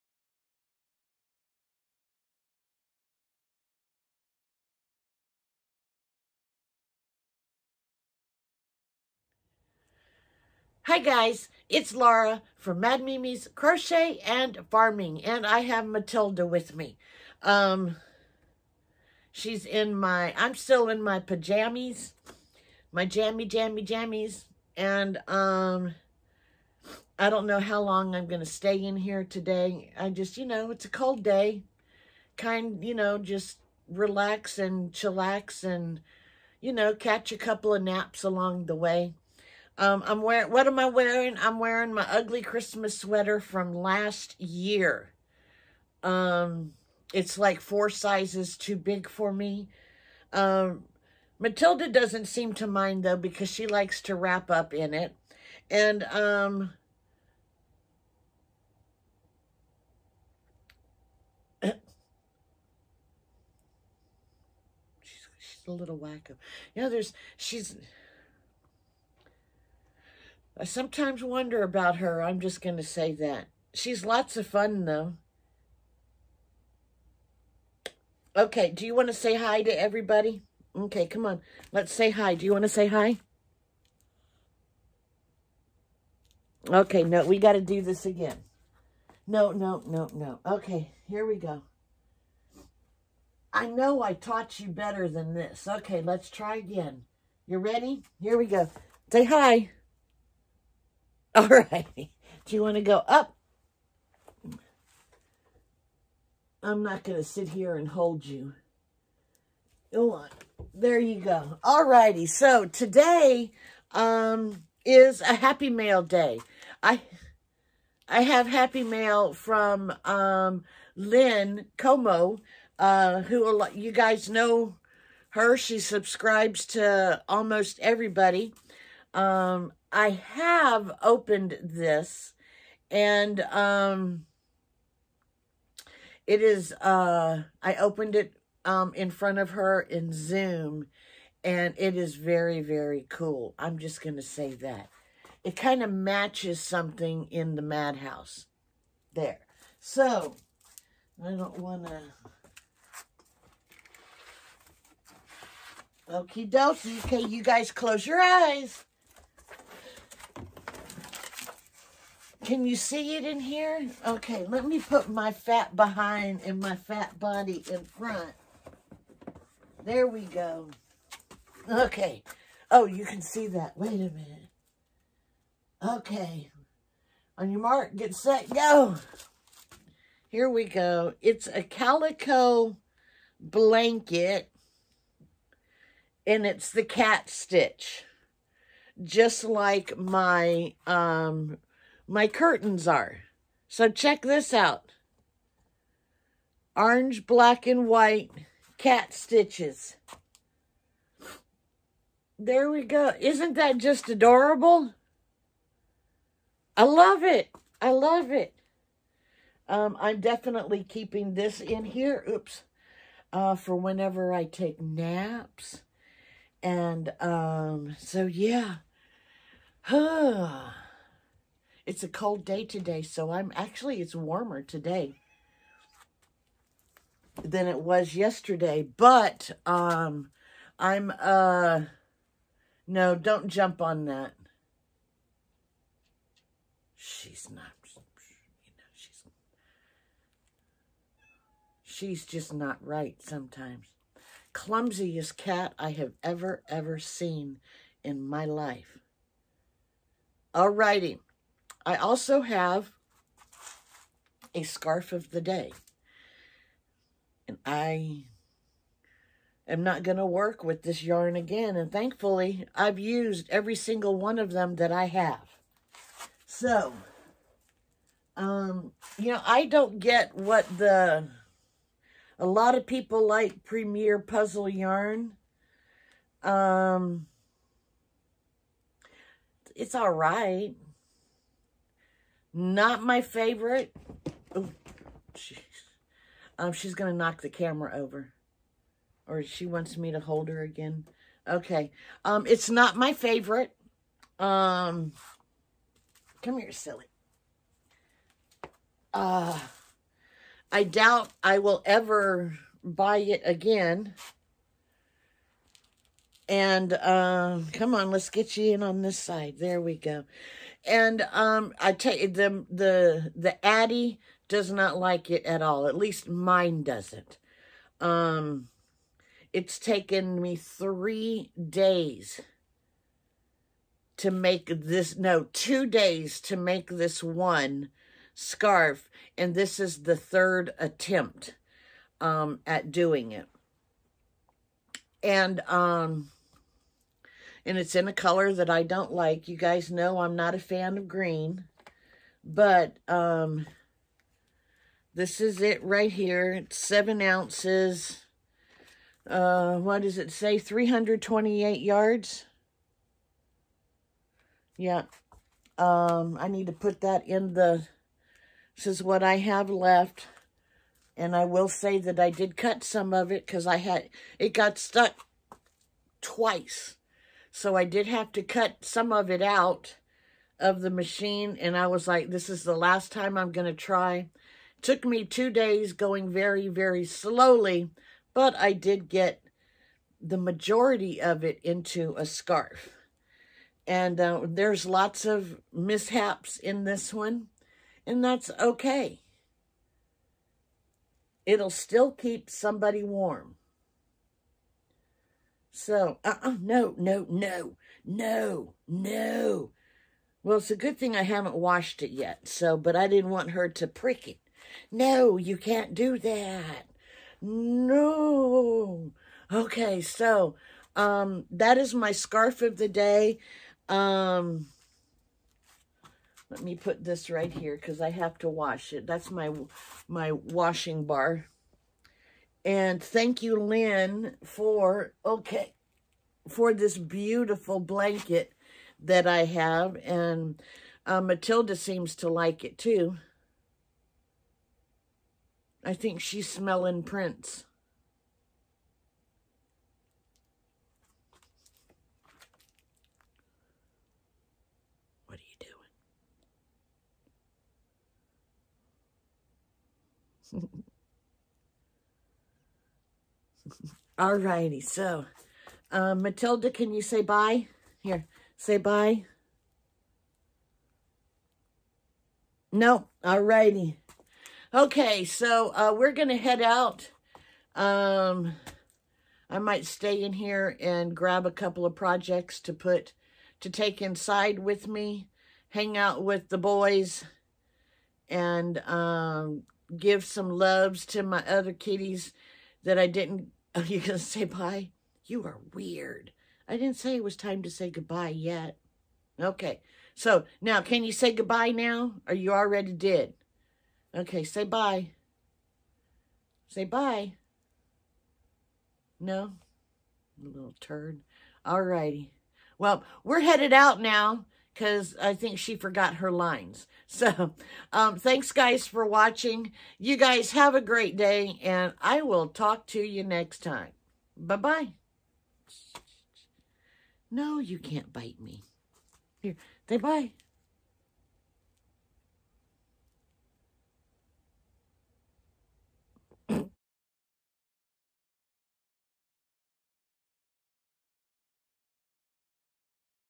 Happy Mail from Lyn
Thank you Lyn @lyncomeaux6718 for the wonderful happy mail. I'm so ready to snuggle under the blanket.

Snail Mail:
Laura McDonald
Mad Mimi’s Crochet